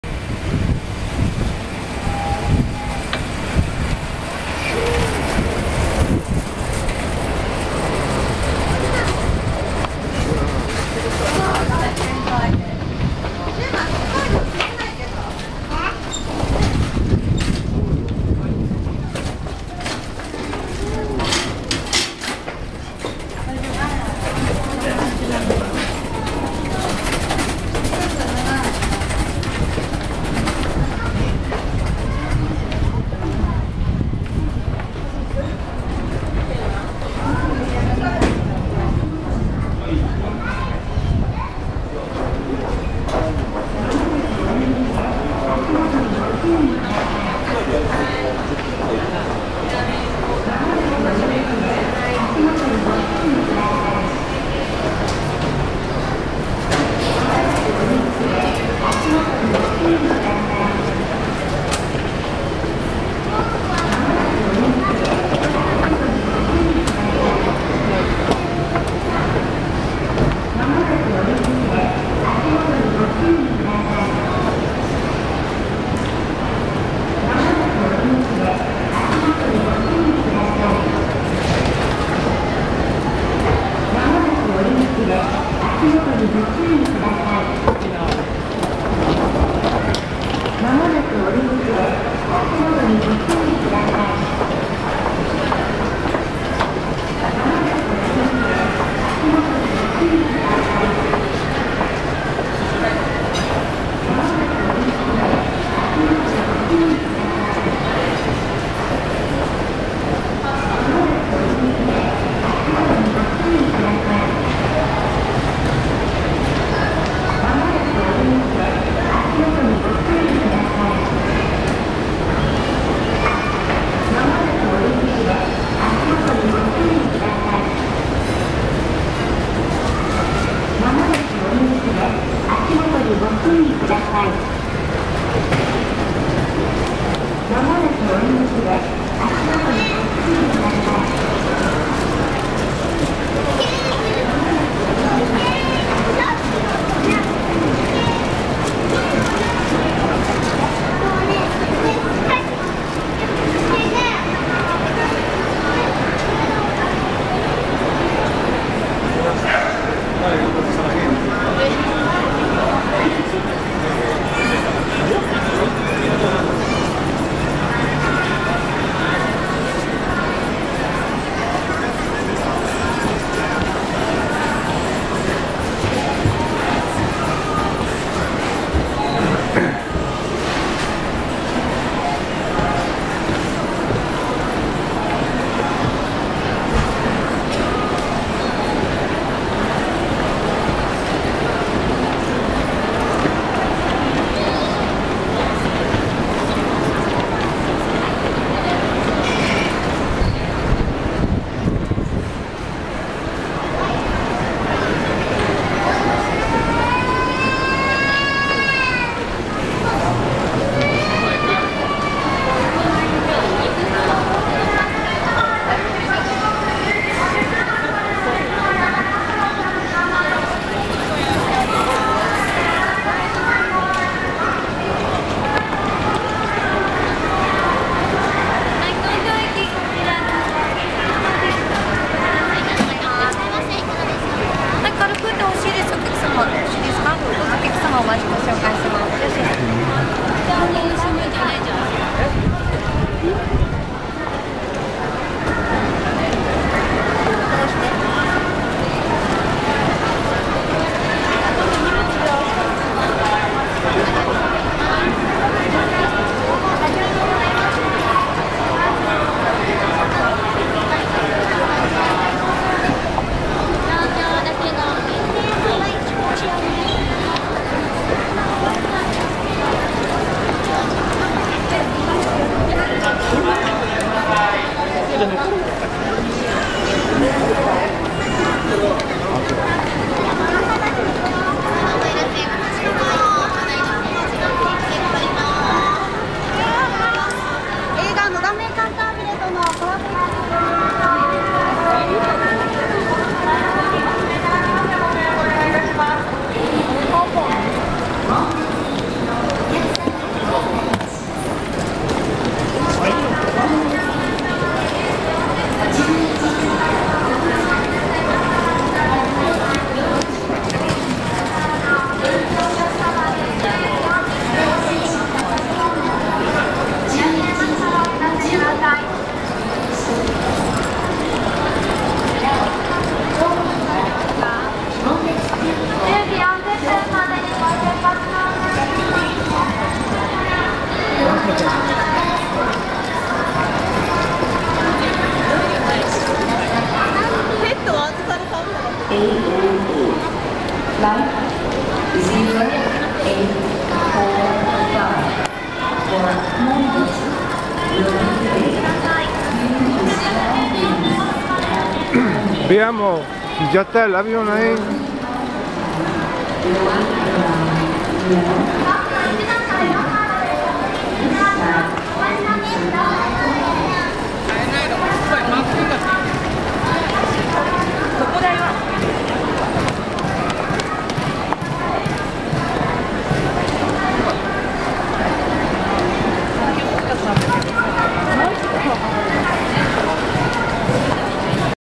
(UNO) Aeropuerto de Haneda Tokio En busqueda de las Raices ( Uno)
Este  es  el  Aeropuerto(Kuko en  japones ) de  Haneda  en  Tokio,   desde  donde  partimos  con  rumbo a  Kagoshima, con  la  intencion  de  conocer  la  tierra  y  el  lugar  donde  nacvio  y  paso  su  juventud,  mi  querido  Abuelo Eisuke  Iwashita.  Como  poodran  ver  es  bastante  grande  y  desde  alli  parten  y  arriban  los  vuelos  nacionales.